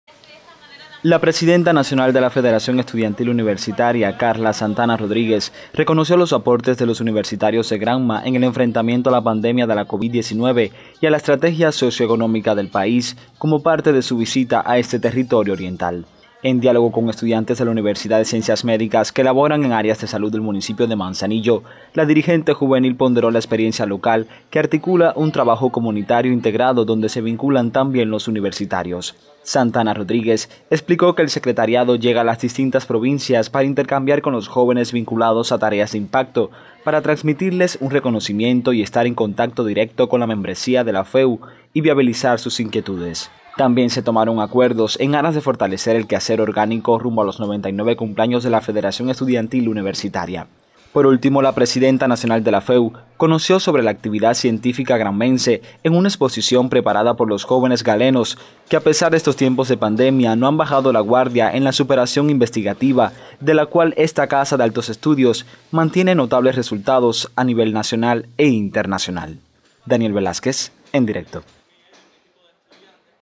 Sostiene encuentro Presidenta Nacional de la FEU con universitarios granmenses
La Presidenta Nacional del Federación Estudiantil Universitaria Karla Santana Rodríguez sostuvo un encuentro con el consejo de la FEU y directivos de la Universidad de Ciencias Médicas de Granma. El intercambio forma parte de un recorrido que realiza por todas las universidades del país. En la casa de altos estudios pudo constatar el acompañamiento y participación de los estudiantes en el enfrentamiento a la COVID-19 y el funcionamiento de la organización.